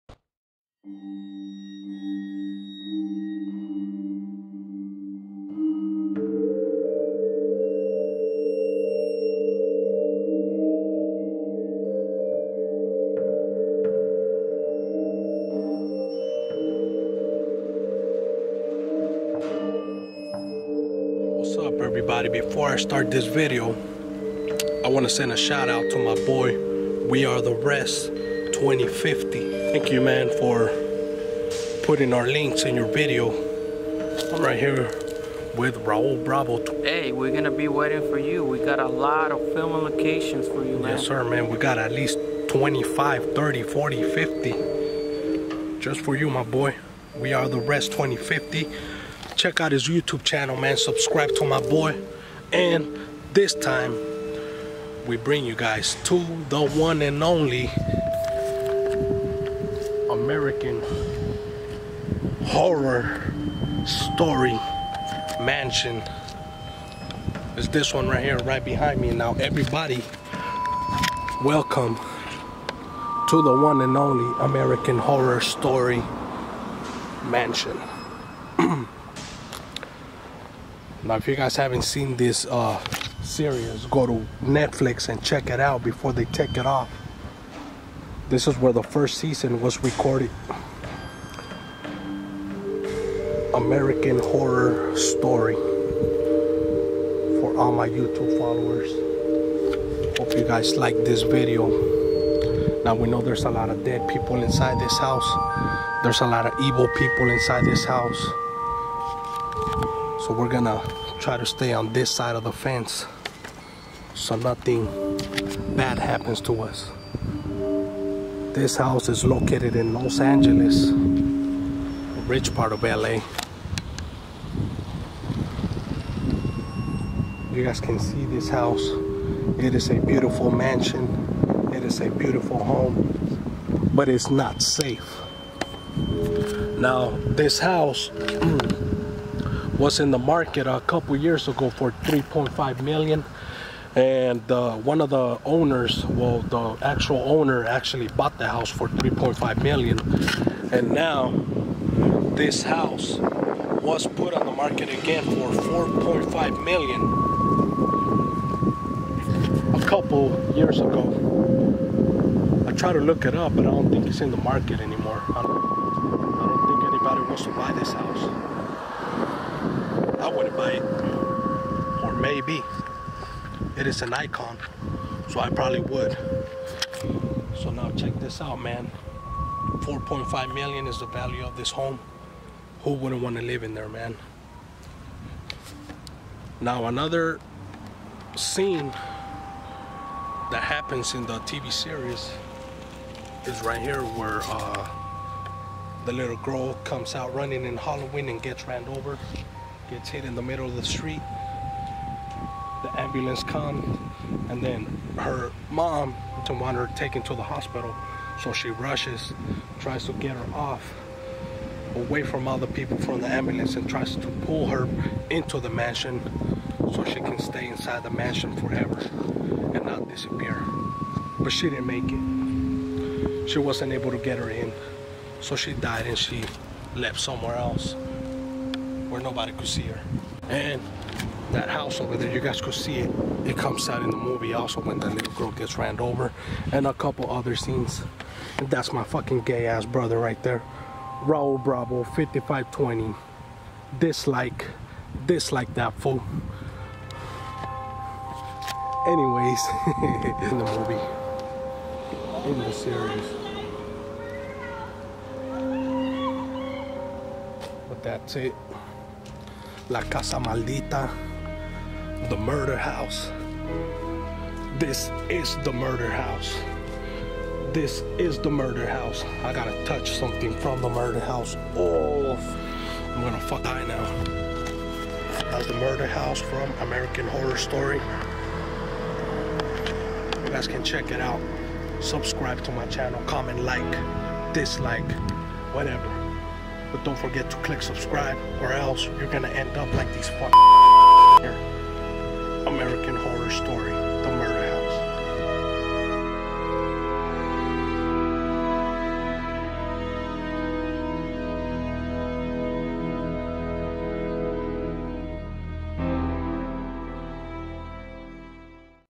American Horror Story (The Murder House)
we went to the murder house from the show that is now playing on Netflix
hope you guys like the video and i leave you guys at the murder house hope you guys make it out alive